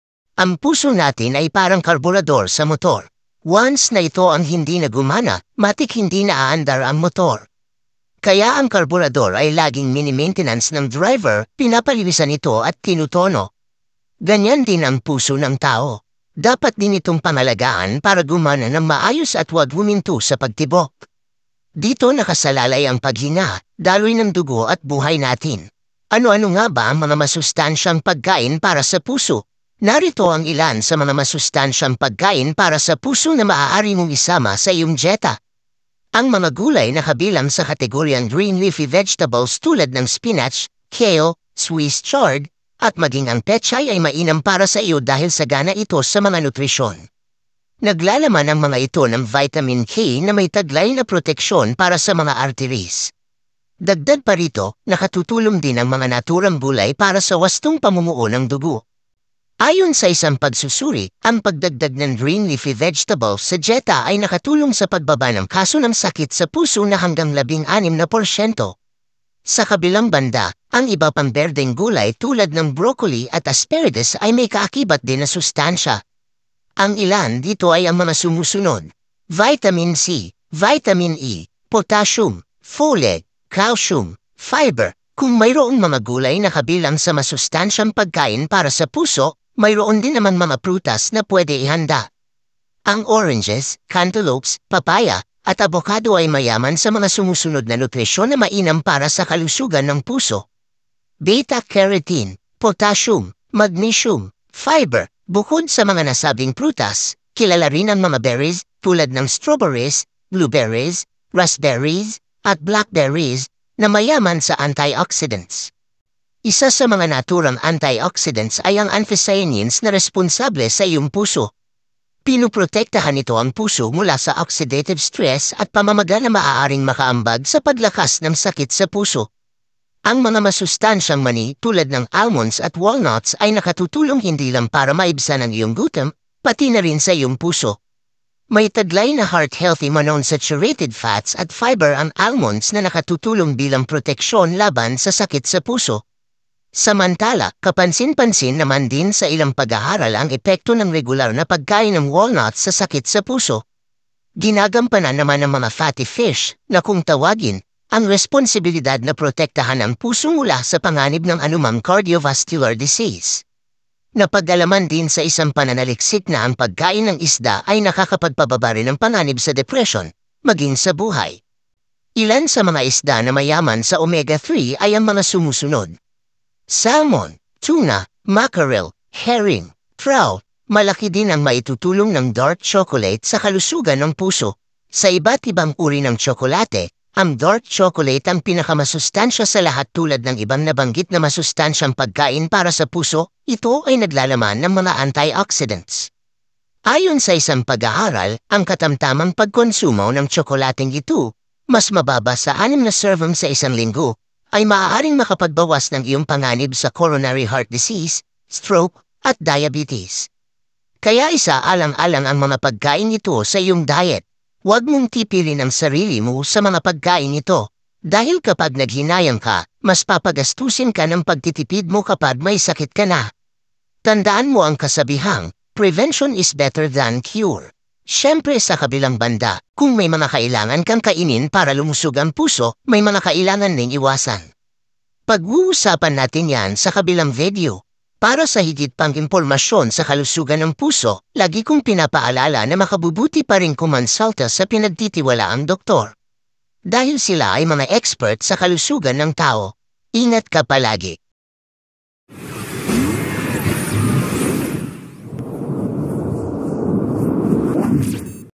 Para lumakas ang puso ito ang mga pagkain na dapat kainin
Ang puso ay parang karburador ng motor, dapat itong may maintainance din para hindi pumalya. Kumain ng mga pagkaing dapat sa puso para maging malusog ito. At para maka iwas o ma prevent ang mga sakit sa puso.